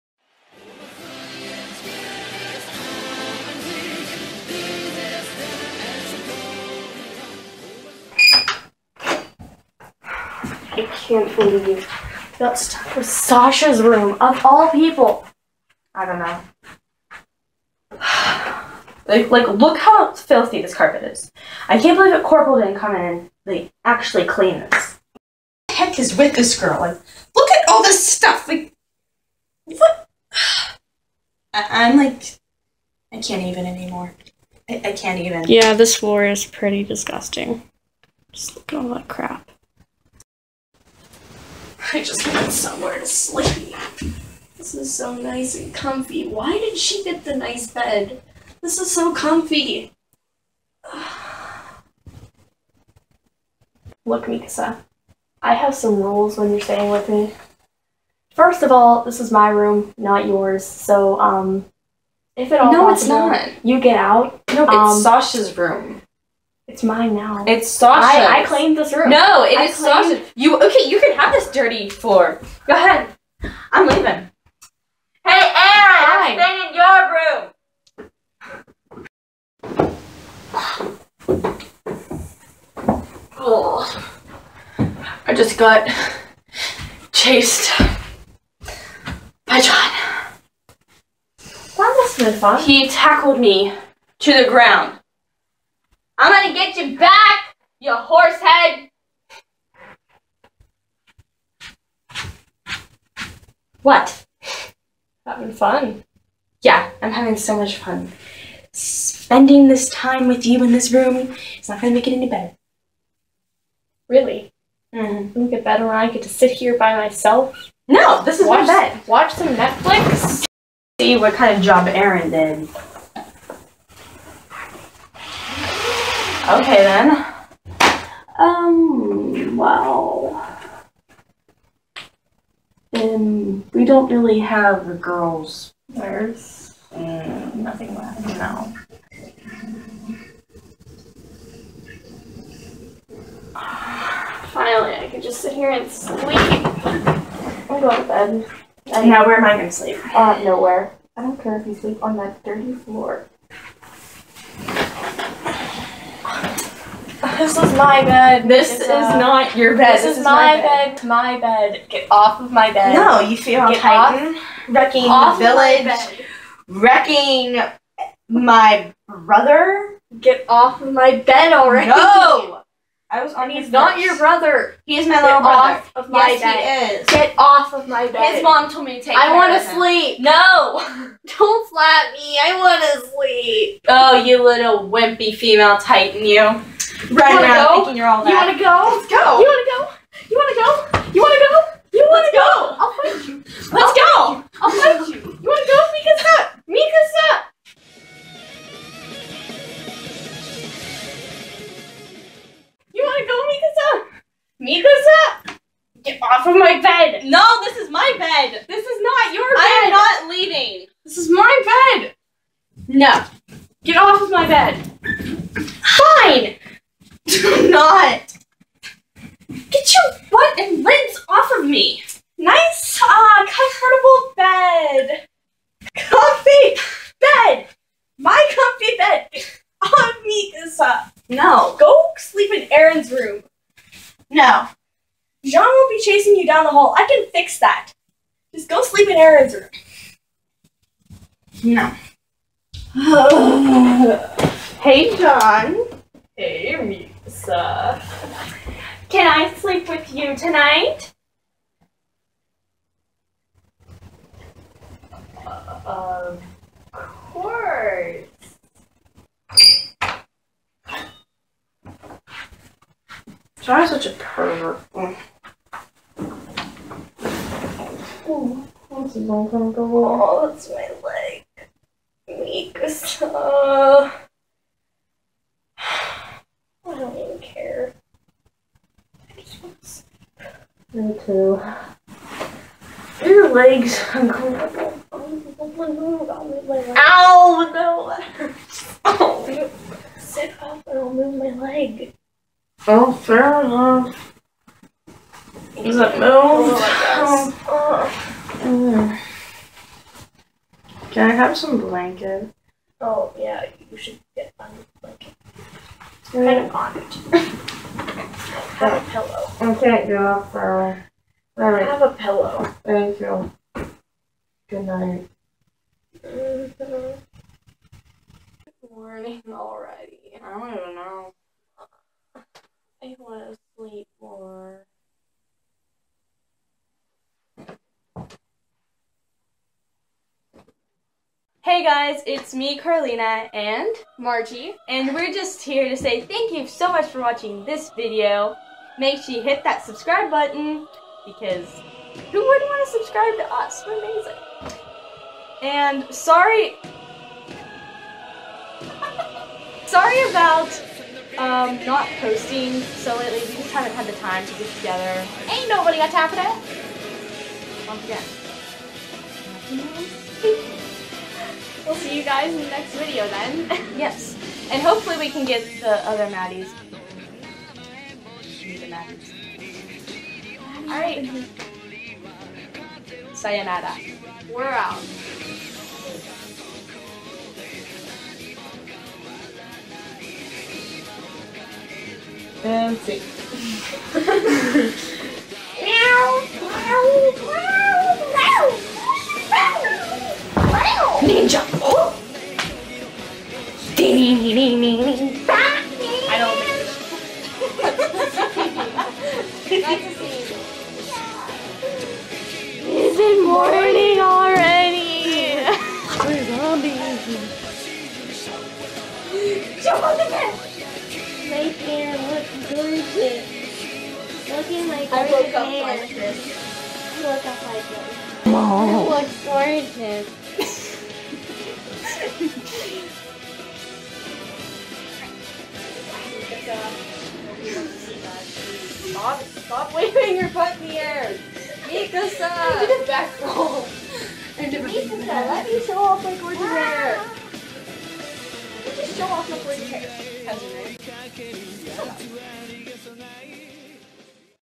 Annie and Mikasa Rooming
After Eren destroys the girls' dormitory, Annie and Mikasa are forced to room together in Sasha's room. Sasha is (obviously) in the kitchen 24/7, so the two girls will have to deal with each other.
Mikasa: Carlina
Annie: Margie
Directing: Margie and Carlina